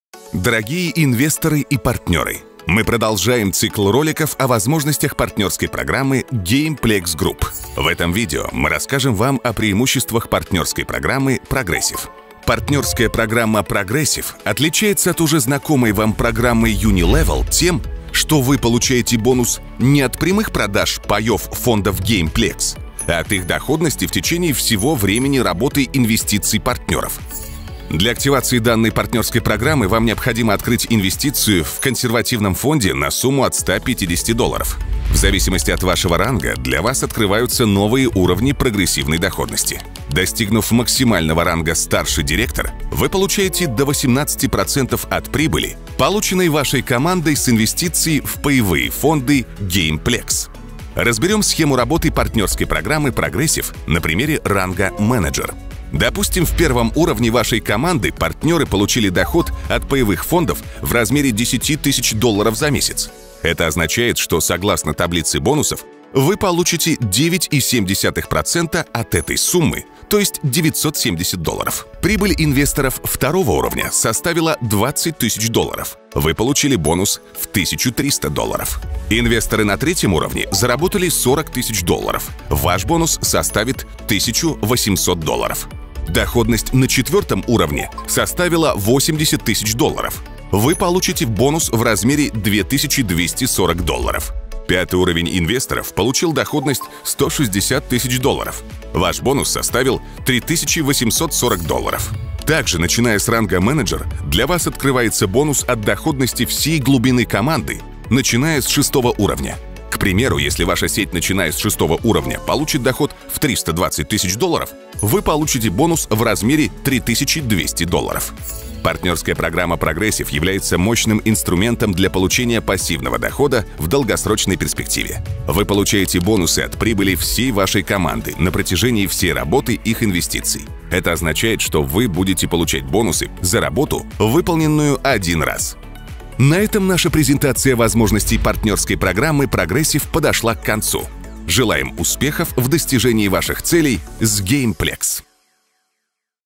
Progressive affiliate program / Партнерская программа Progressive (RUS)
В этом видео вы ознакомитесь с работой партнерской программы Progressive.